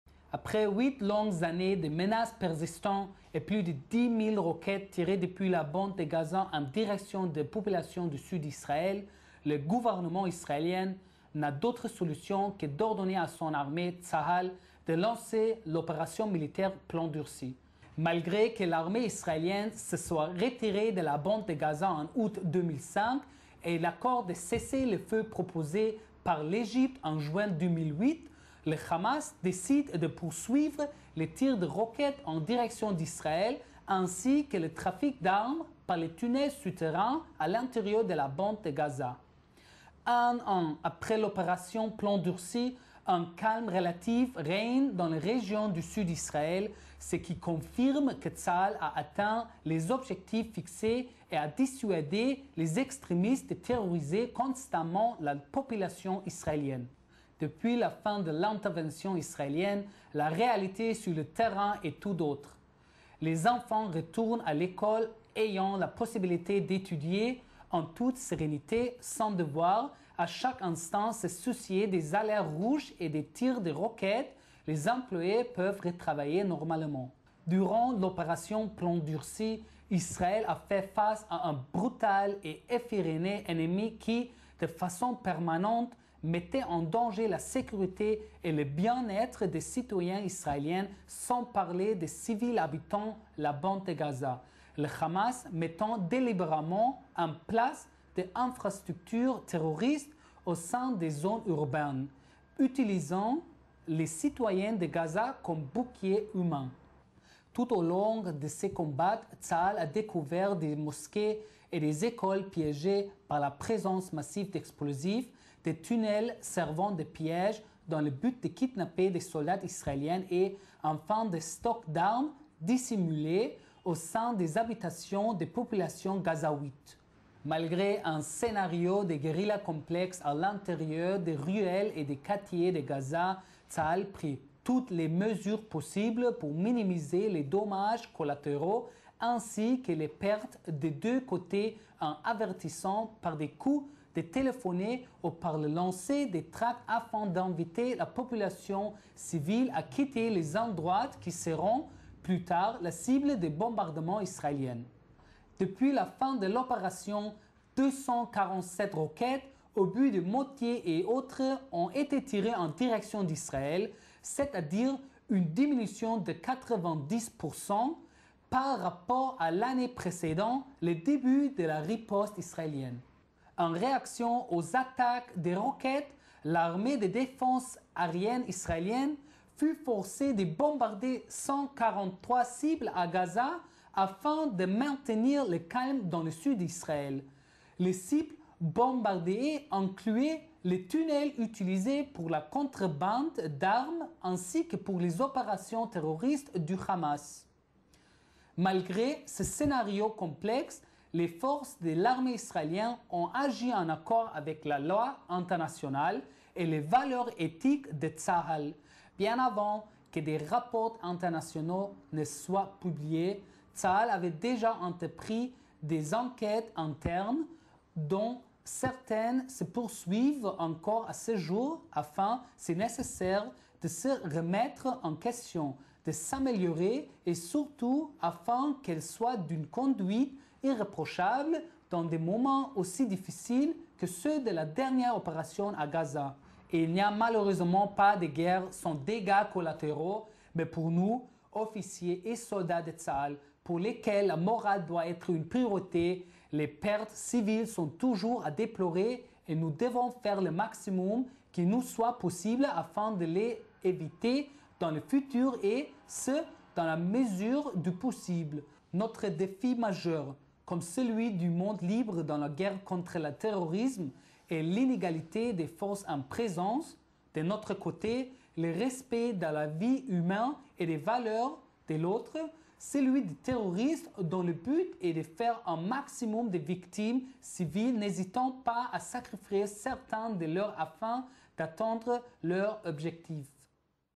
VLOG: Le Capitaine Arye Sharuz Shalicar an l'Operation Plomb Durci
Le Capitaine Arye Sharuz Shalicar  s'adresse au monde un an apres la fin de l'Operation Plomb Durci. Malgre le retrait de Gaza par Israel et le cessez-le-feu obtenu par l'intermediaire de l'Egypte, le Hamas a continue a lancer des attaques a l'encontre des civiles israeliens. Une fois que le Hamas ait reussi a mettre un million de civils israeliens sous la menace des tirs de roquettes, Israel n'a eu le choix que de lancer une offensive le 28 decembre 2009.

Confrontee a un ennemi qui place intentionnellement sa propre population en situation de danger, l'armee israelienne fit tous les efforts pour proteger les vies de civils innocents. Des milliers de SMS ont ete envoyes, des millions de tracts ont ete lances ainsi qu' au moyen des chaines de television et radios palestiniennes, Tsahal a fait tout son possible pour limiter les dommages civils en depit de la strategie du Hamas.

Depuis la fin de l'Operation Plomb Durci, le nombre de roquettes tirees en direction du territoire israelien a baisse de 90%. Cela prouve bien le succes de l'Operation dans son but de reinstaurer le calme dans les villes israeliennes. Depuis un an, les enfants du sud d'Israel peuvent aller a l'ecole sans se soucier de savoir ou se trouve l'abris a bombe le plus proche pendant que les commerces reprennent pleinement leur activite.